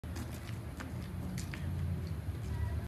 Windsor Spitfires bus spotted in Peterborough
The Ontario Hockey League, Windsor Spitfires team bus spotted in Peterborough at the Memorial Centre home of the Peterborough Petes.